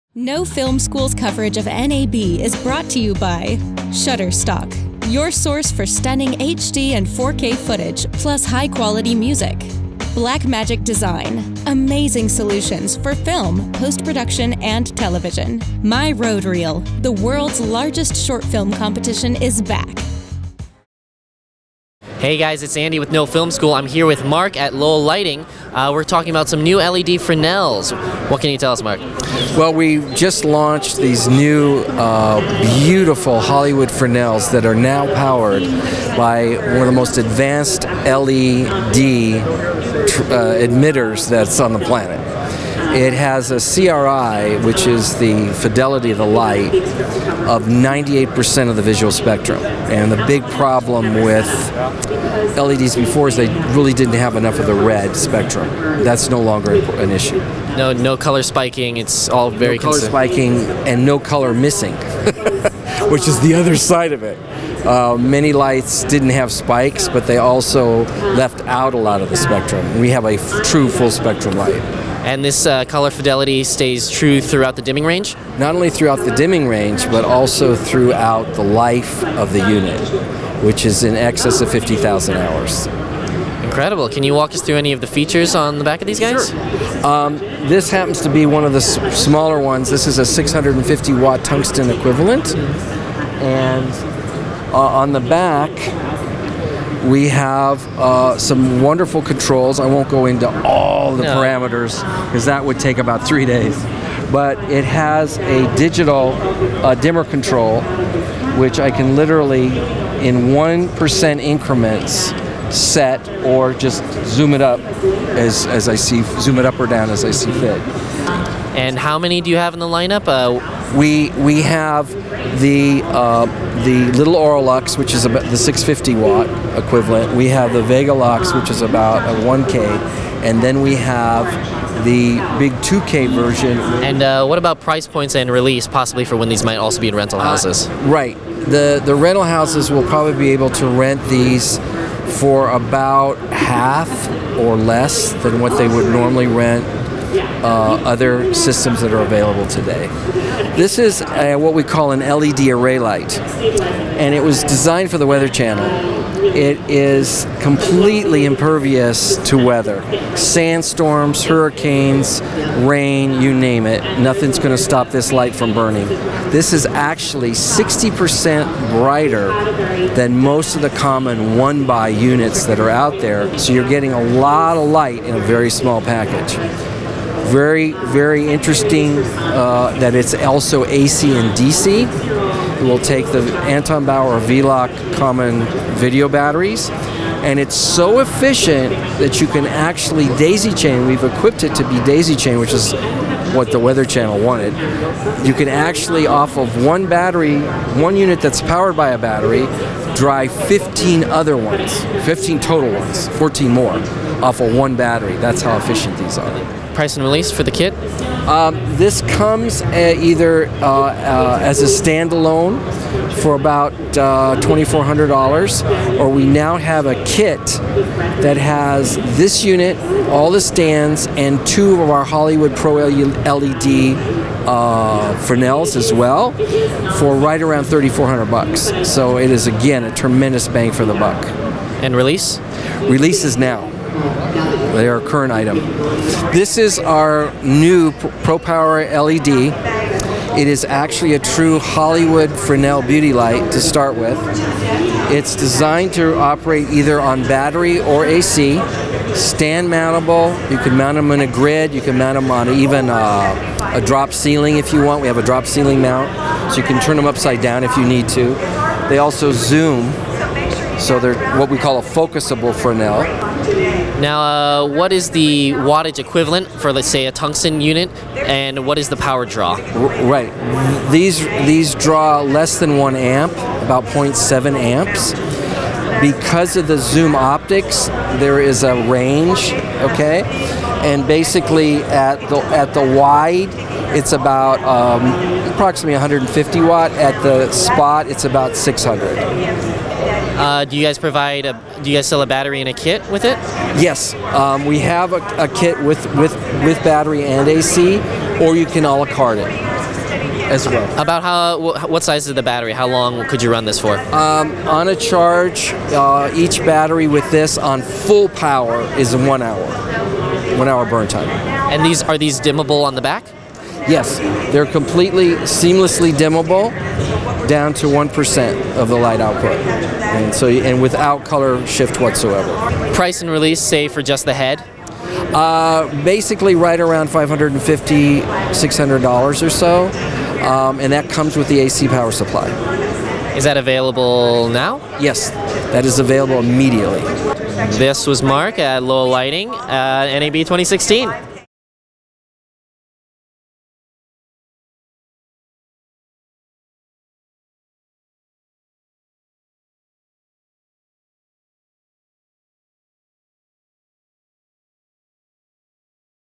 Lowel's New LED Lights Minimize Nasty Color Spikes
Lowel walked No Film School's Andy Zou through their new line of lighting products at NAB 2016. They were quick to show off their new models of LED Hollywood Fresnels, which they claim have a Color Rendering Index (CRI) of 98% of the visual spectrum. You'll be able to find these lights in a few different models once released: the smaller, 650 watt Aura Lux as well as the larger, 1k and 2k watt Vega Lux.

They also introduced us to a new, weather resistant LED array light, designed specifically with parameters from The Weather Channel in mind, and an upgraded model of the Pro Power LED.